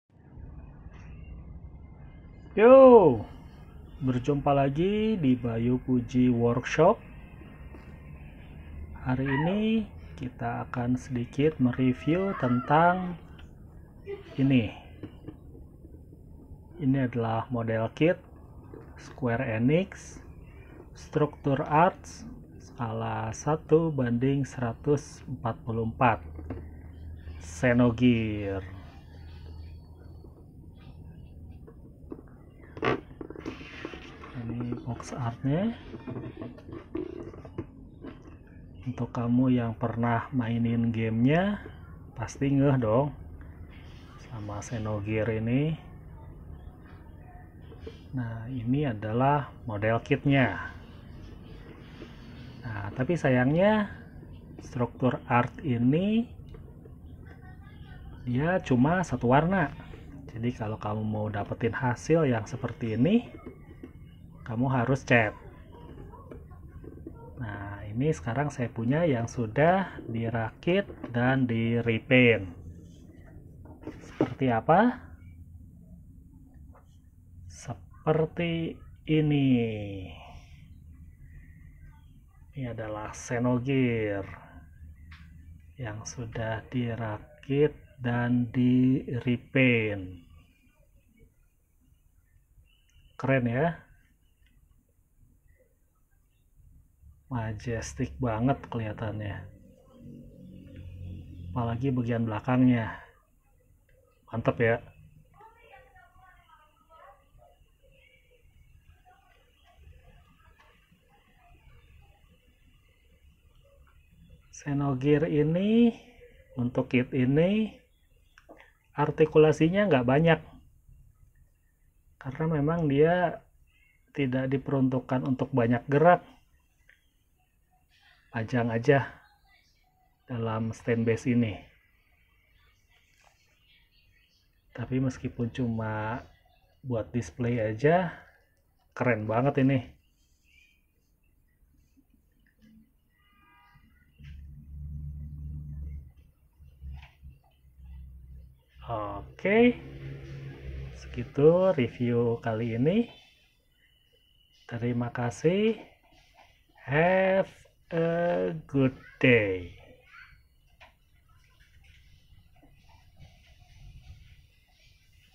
(English subtitle) Model kit Square Enix 1/144 scale Structure Arts Xenogears assembled and painted
Di video kali ini BayuPuji Workshop mereview tentang Model kit Square Enix skala 1/144 Structure Arts Xenogears yang sudah dirakit dan direpaint
Silakan disaksikan video-nya
Klo ada yang ingin ditanyakan, tidak usah sungkan, silakan

(English ver.)
In this video, BayuPuji Workshop is reviewing Model kit Square Enix 1/144 scale Structure Arts Xenogears assembled and painted
Feel free to watch the video